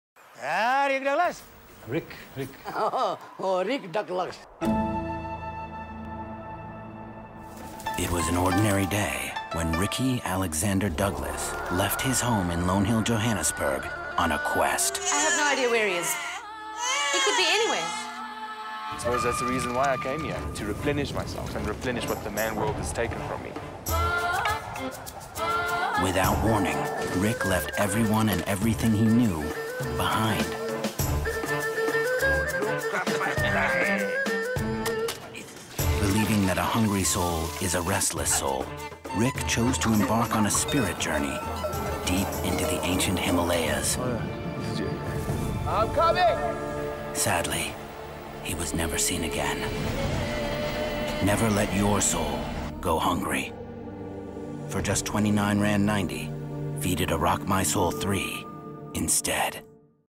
MarkLives AdoftheWeek 2014: Chicken Licken's Rick — Net#work BBDO & Plank
by Oresti Patricios. In “Rick”, Chicken Licken continues with its absurdist vein of humour, telling the story of a very ordinary office worker who abandons his job, his Lonehill Johannesburg home, his wife and baby, to go ‘on a quest’. The entire video was shot by Net#work BBDO in Nepal, with locals for extras, for the sake of authenticity. The story is told by a narrator, interspersed with comments from various people.

Ad of the Week, every Wednesday on MarkLives.com